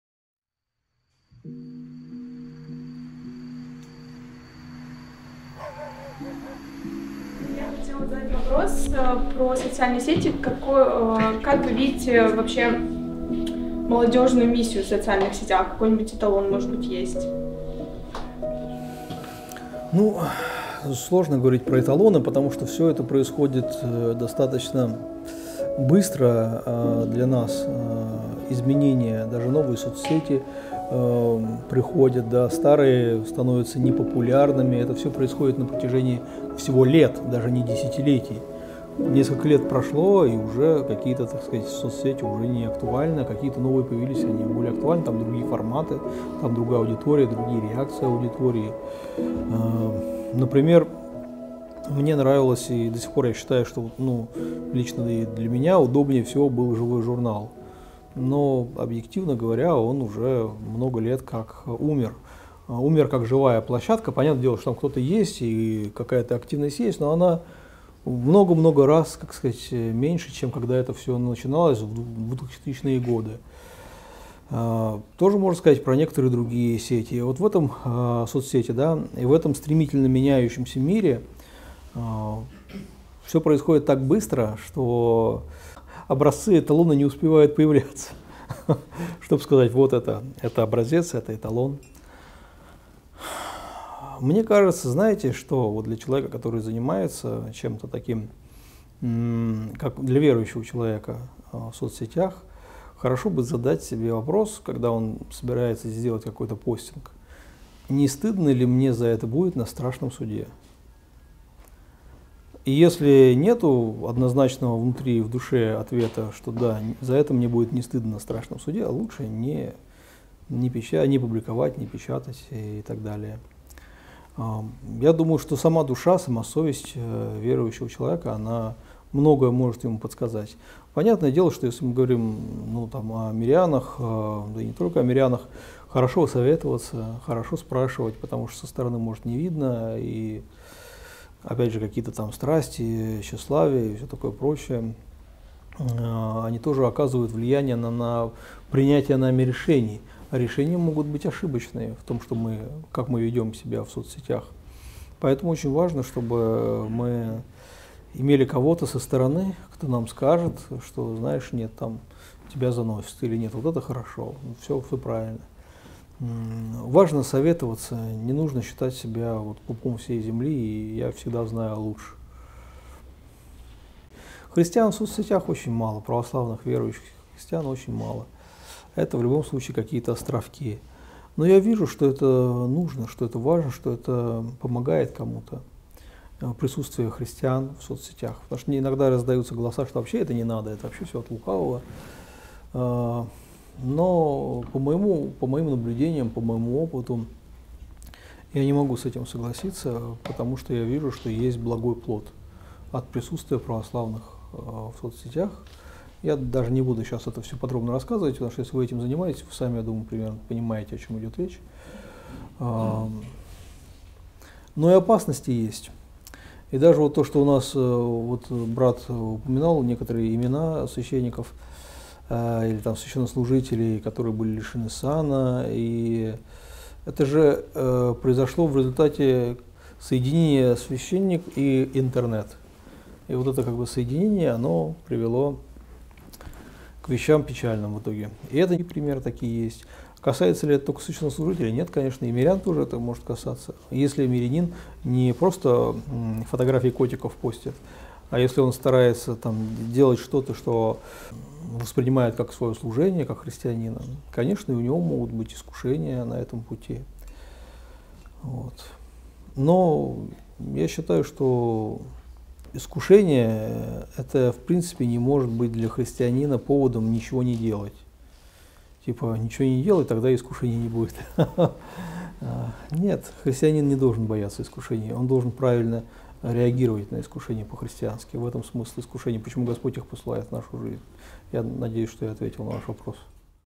Христианин в соцсетях: мое мнение
Ответ на вопрос во время встречи в храме свт. Спиридона Тримифунтского (Краснодар)